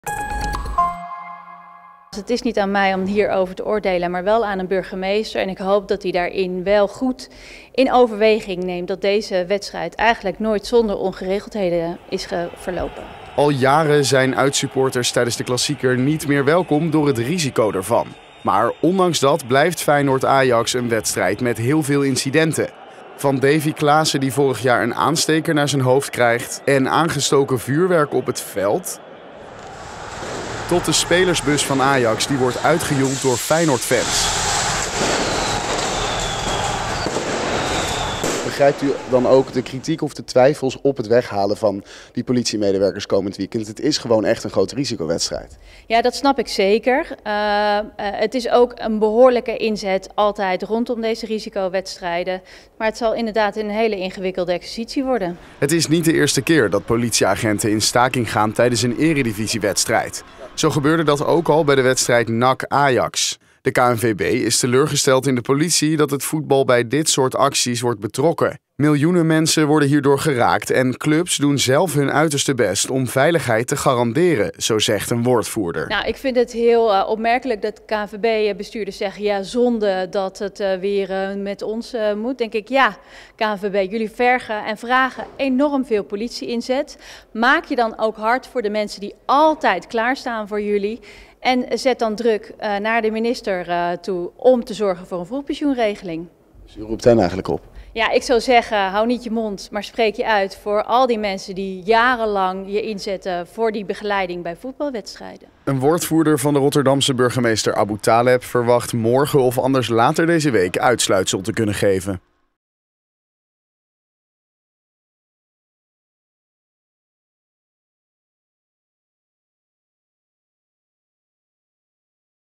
Politie staakt tijdens Klassieker
Tijdens de wedstrijd tussen Feyenoord en Ajax in Rotterdam komend weekend zal er geen politie ingezet worden. Meerdere politiebonden voeren actie voor een verbeterd vroegpensioen. De afgelopen weken werd er ook al bij andere eredivisiewedstrijden gestaakt en daar komt nu De KNVB reageert teleurgesteld.

Volgens de politiebond zijn er vanuit de regering 'geen signalen gekomen dat er wordt gewerkt aan een verbeterd vroeg pensioen'. Politiemensen verzamelen zich tijdens de wedstrijd op een speciale vakbondsbijeenkomst die op afstand van De Kuip wordt georganiseerd.

KNVB teleurgesteld
Of dit betekent dat de wedstrijd verplaatst moet worden, is nog onduidelijk. "Wij hebben nog niet vernomen dat de wedstrijd zondag definitief niet kan doorgaan", zegt de KNVB tegenover RTV Rijnmond.

Wel zegt de KNVB teleurgesteld te zijn over het besluit van de politiebond: "Wij zouden het echter enorm betreuren als - helaas niet voor het eerst - het voetbal hiervoor wordt gebruikt en, zeker in het geval van de Klassieker, niet alleen de betrokken clubs, maar miljoenen mensen er door worden geraakt die nu al enorm uitkijken naar deze wedstrijd."

Nine Kooiman, voorzitter van de Nederlandse Politiebond, vindt dat een opmerkelijke reactie. "Dan denk ik: jullie vergen en vragen enorm veel politie-inzet. Maak je dan ook hard voor de mensen die altijd klaarstaan voor jullie."

Eerst NAC, nu Feyenoord
Vorige week was er rondom de uitwedstrijd tegen NAC ook geen politie-inzet. De wedstrijd ging ondanks de politiestaking wel door. Burgemeester Paul Depla van Breda gaf toen al aan dat dat alleen kon omdat wedstrijden tussen NAC en Ajax normaal gesproken goed verlopen. "Anders had ik de wedstrijd moeten verbieden", zei hij tegen de NOS.

De wedstrijden tegen Feyenoord zijn veel meer beladen. Vanwege ongeregeldheden rondom supporters zijn er sinds 2009 geen uitsupporters meer welkom bij wedstrijden tussen Ajax en Feyenoord. Ook met die maatregel liep het de afgelopen jaren een aantal keer uit de hand. Zo zochten Ajax-supporters vorig seizoen nog de confrontatie met de ME, nadat ze even daarvoor vuurwerk op het veld hadden gegooid.

Ook in Rotterdam kunnen Klassiekers ontsporen. Sinds Davy Klaassen twee seizoenen geleden een aansteker tegen zijn hoofd gegooid kreeg, hangen er rond elke wedstrijd tegen Ajax netten tussen het veld en de tribunes. Dat zal komend weekend ook het geval zijn. De wedstrijd werd aan het begin ook al onderbroken omdat er vuurwerk op het veld gegooid werd.

Spelersbus
Rondom andere wedstrijden in De Kuip was het evengoed onrustig. Veel van die incidenten speelden zich buiten het stadion af. In december 2021 verzamelden zich honderden supporters bij de Kuip om de bus van de Ajax-selectie op te wachten. De supporters gooiden toen met zwaar vuurwerk naar de spelersbus van Ajax en naar de politie. Een van de fans gooide een baksteen door de voorruit van een politiebusje.

In 2023 werden de spelers van Ajax in een 'anonieme' bus naar De Kuip gebracht. Tijdens de vorige editie stuurde Ajax een lege bus vooruit om Feyenoordsupporters op het verkeerde been te zetten. Een herkenbare Ajax-bus kon vervolgens zonder problemen bij het stadion aankomen.

Een woordvoerder van burgemeester Ahmed Aboutaleb van Rotterdam laat weten dat er op dit moment geen beslissing is genomen over het al dan niet door laten gaan van de wedstrijd. Later op de dag moet daarover meer duidelijk worden.